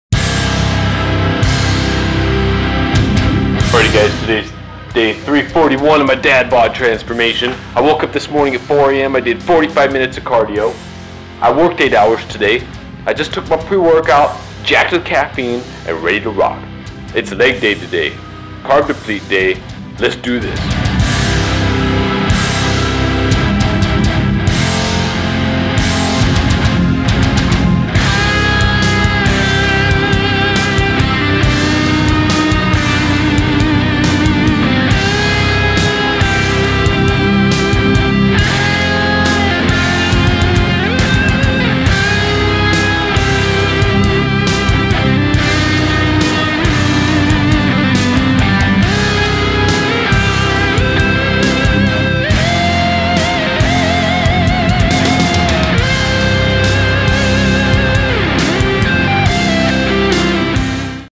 LEG WORK. Dadbod Transformation. 1 year Transformation. Dadbodinator. John Blade. WHOOAA
Today is day 341 of my Dadbod Transformation.  "25" days to "Ripped Dad"
Leg Workout today.
Todays workout:
1: 2 sets of Belt Squats
2: 5 sets of Levergym Squats
3: 3 sets of Leg extensions + Drop Set
4: 4 sets of Leg Curls
5: 4 sets of Glute Kickbacks
6: 3 sets of 20 calve raises
7: 10 min. Heavy Bag work(aerobic exercise)

Stay the course...Dont give in to temptation, diet cheating, or skipping exercise.  You will feel so good in the end. ..and Look FANTASTIC!